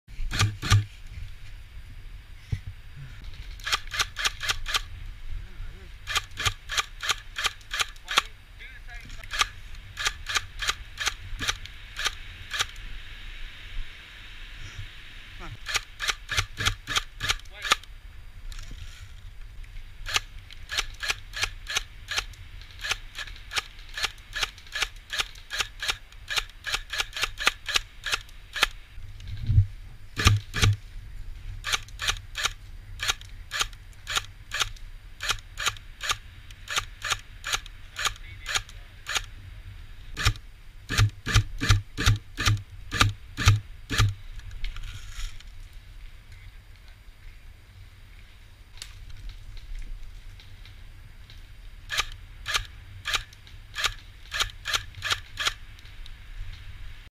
defend the hill lindsey airsoft footage 26/03/16
In this game my team has to defend the hill to win